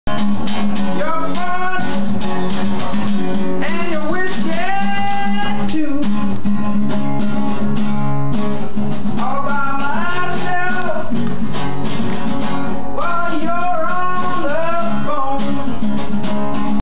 Ryan Kitson was super Americana in 2001
throwing down at Dean Street
first winter in NYC